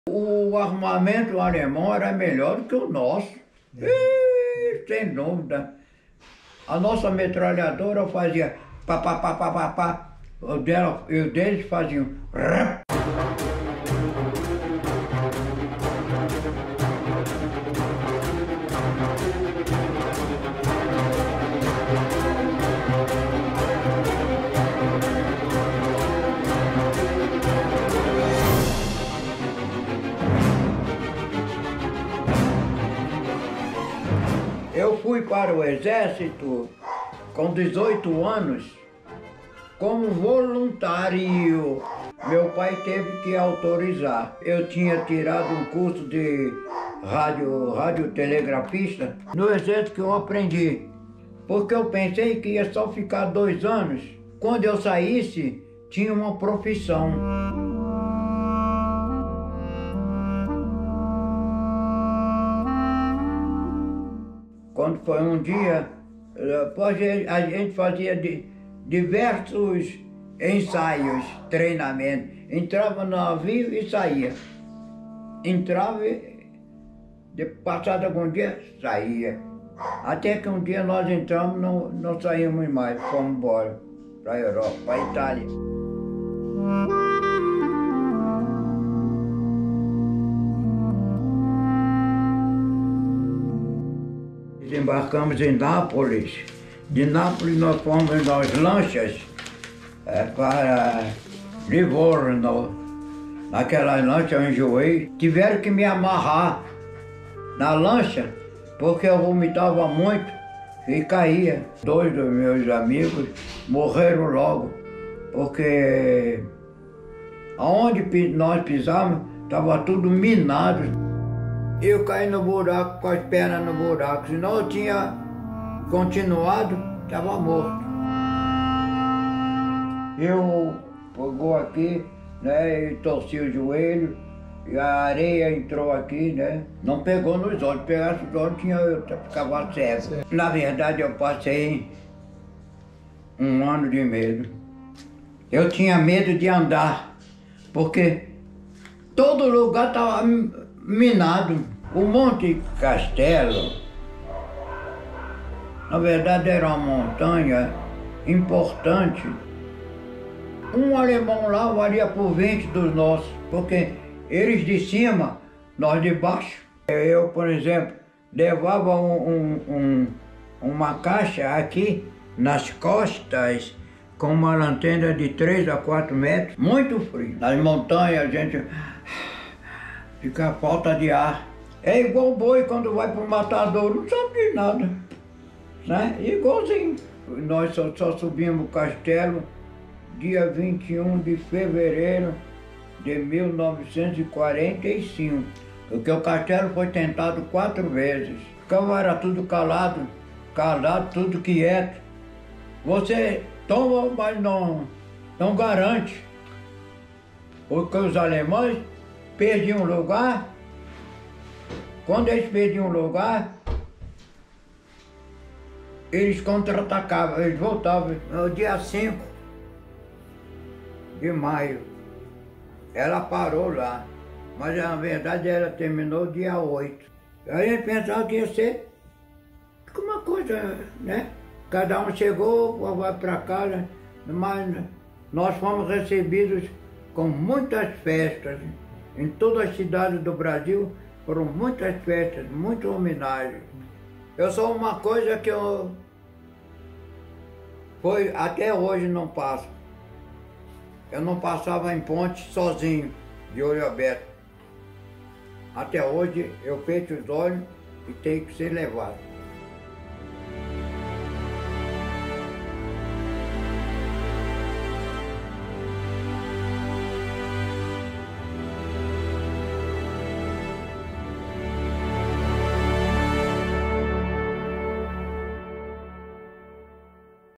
Conquista de Monte Castello, importante batalha da Segunda Guerra, completa 75 anos
O veterano Vasco Duarte Ferreira fala sobre sua experiência na Segunda Guerra Mundial, dando destaque à sua participação na Batalha de Monte Castello. Brasileiros derrotaram as forças alemãs no local emo dia 21 de fevereiro de 1945, após três meses de luta.